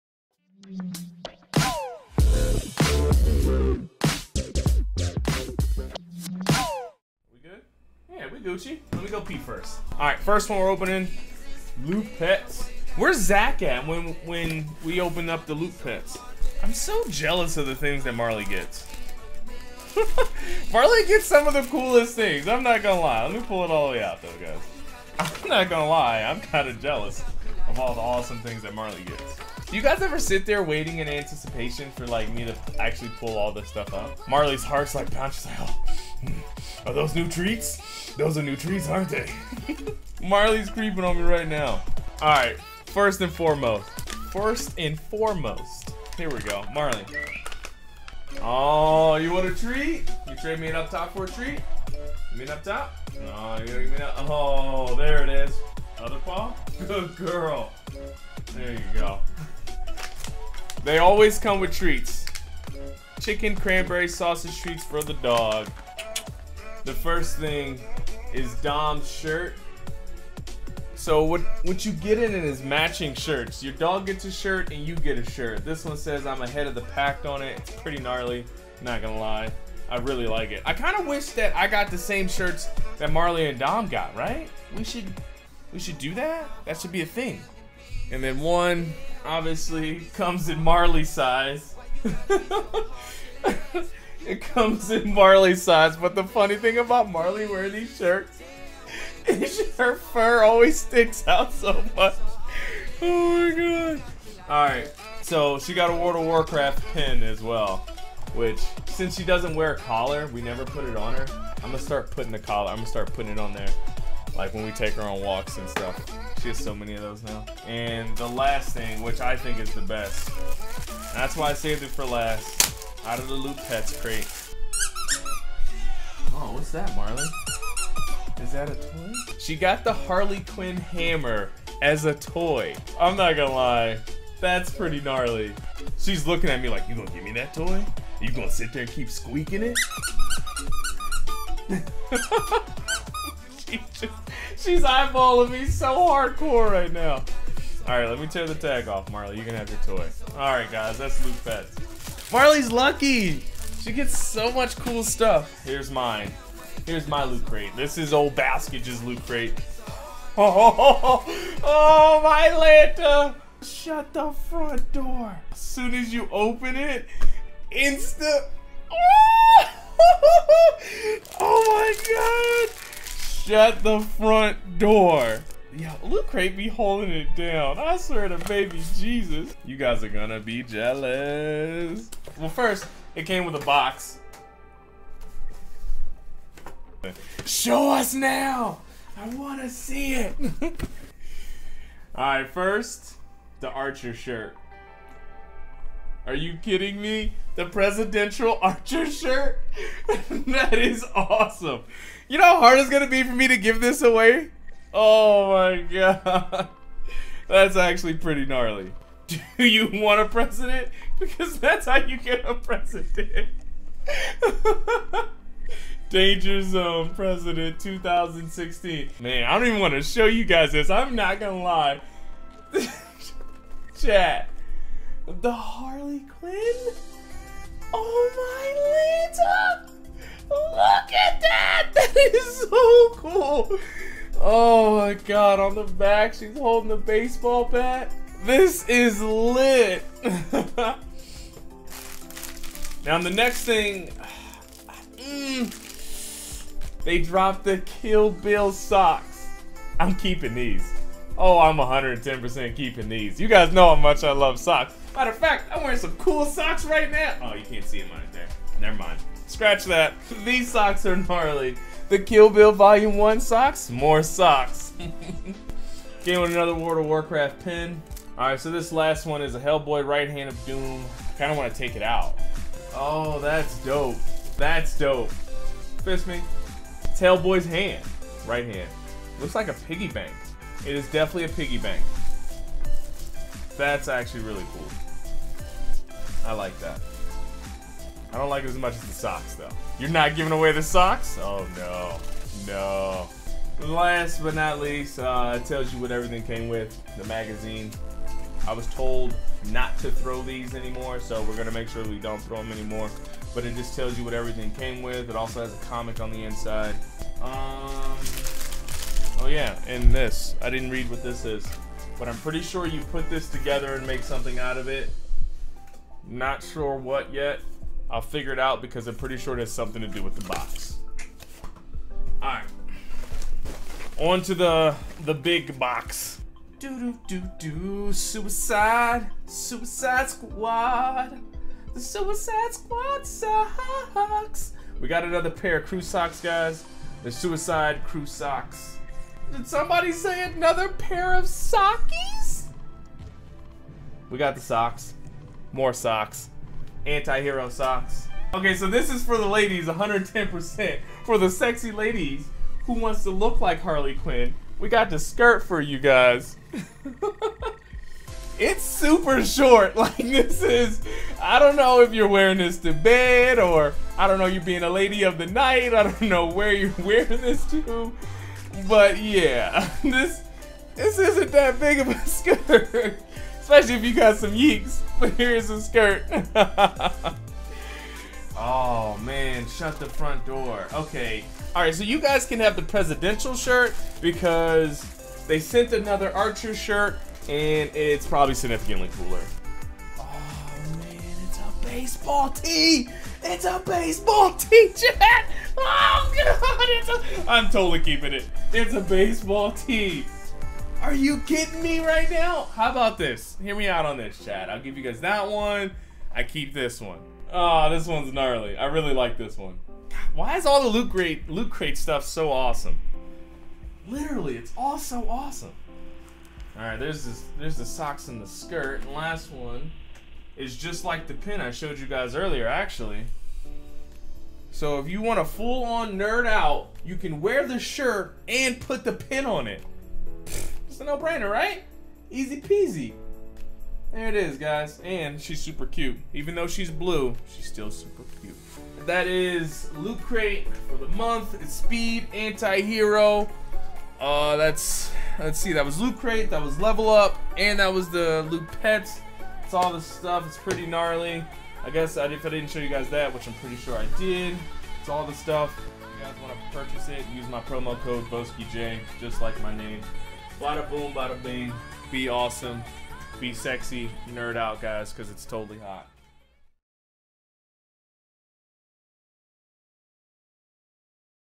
Lootcrate Unboxing August 2016 Anti-hero
Here is an unboxing of the August 2016 lootcrate Anti-hero addition.  All prices listed below.

► Prices
LootCrate - $13.95
LootPets - $19.99
Loot Socks - $9.99
Loot Tees - $8.99
Loot Wearables - $14.99
Loot For Her - $14.99